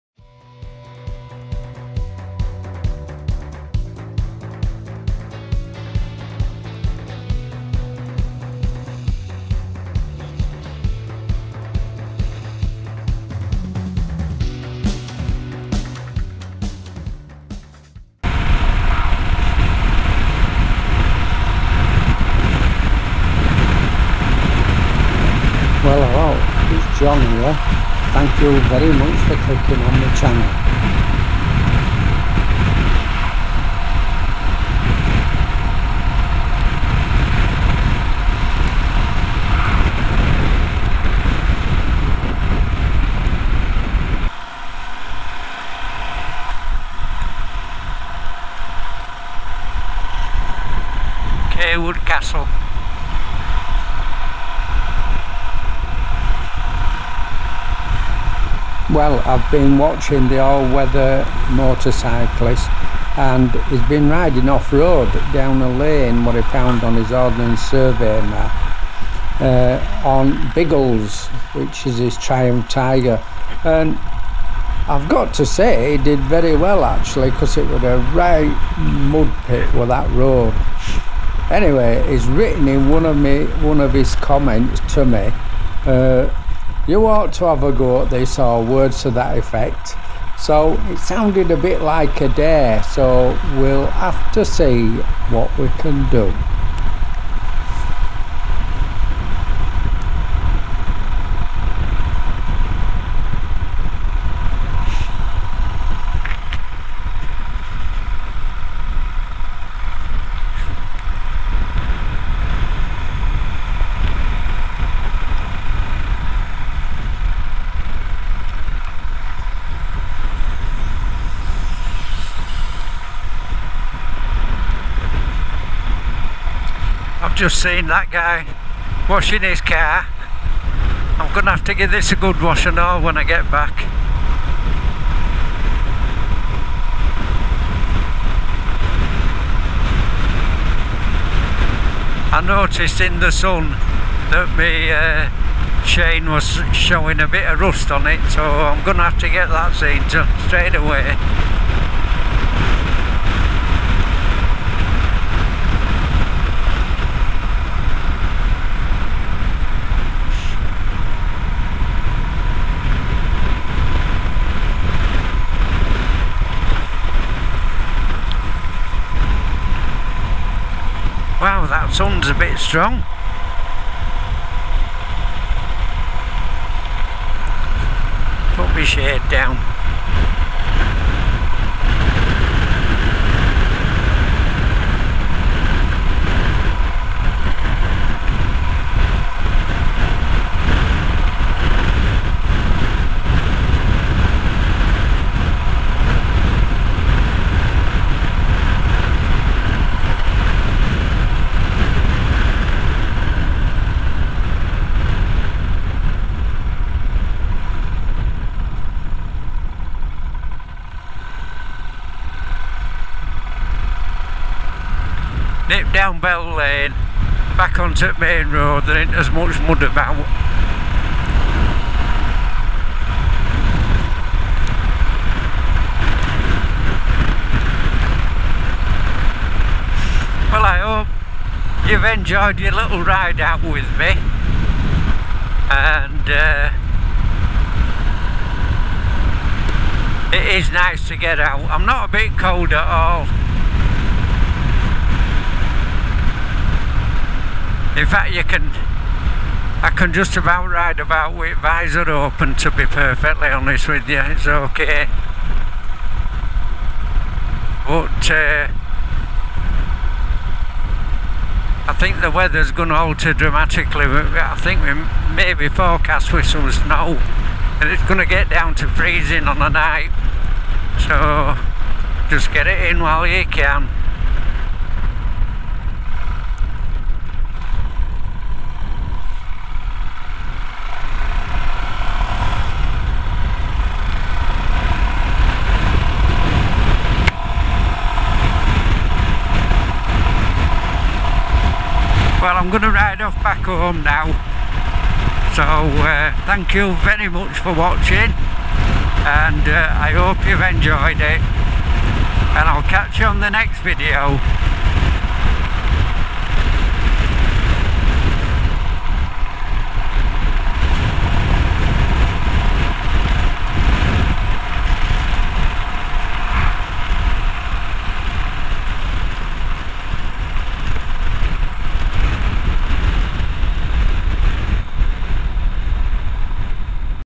Royal Enfield Himalayan Bimble
A little ride on the Royal Enfield near Cawood